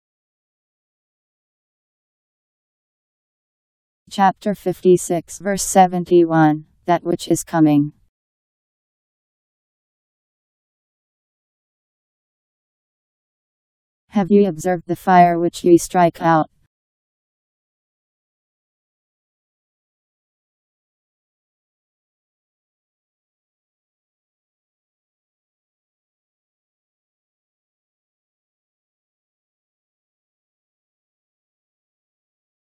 Quran That Which is Coming 56:71
Video shows Quran That Which is Coming Chapter 56 Verse 71: Have ye observed the fire which ye strike out; Powered by MaryTTS. Holy Qur'an 56:71 Surah : Ayah